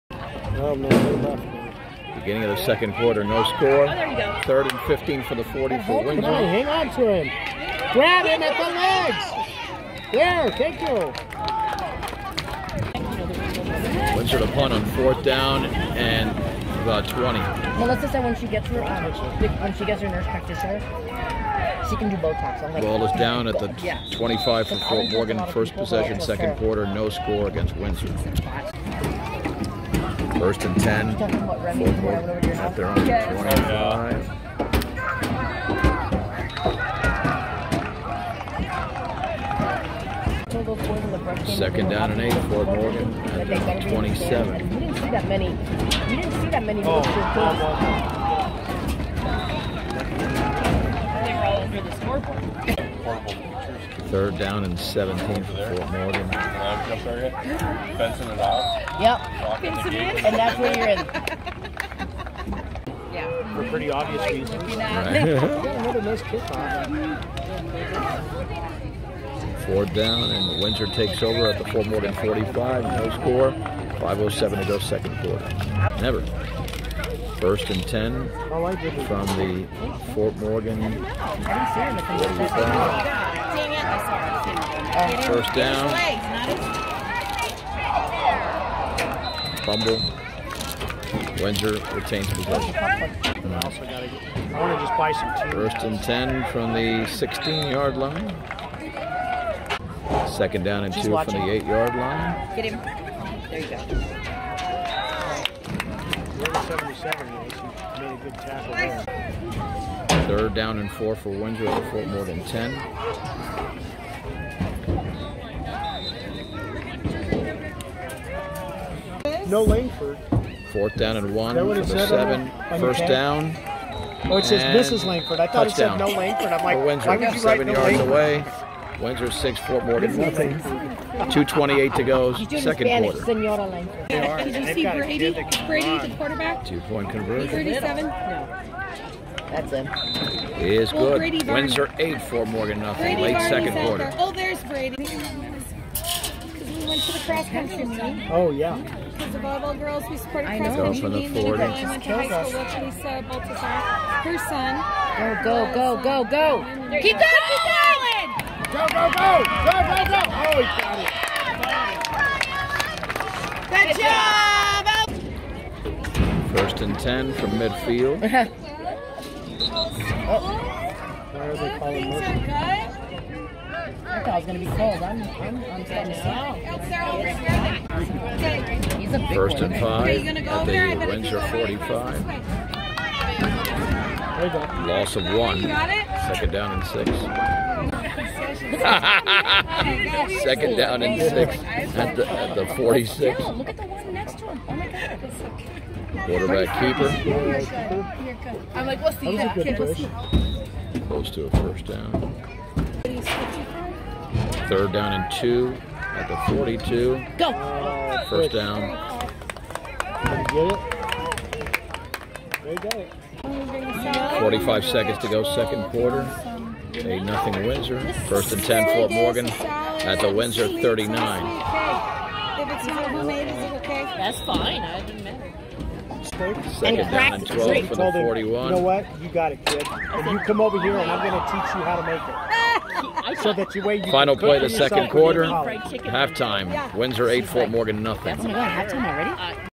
9-14-19 - Fort Morgan 8th grade football vs. Windsor (2nd quarter)
Fort Morgan 8th grade football vs. Windsor on September 14, 2019. Windsor defeated Fort Morgan, 23-0.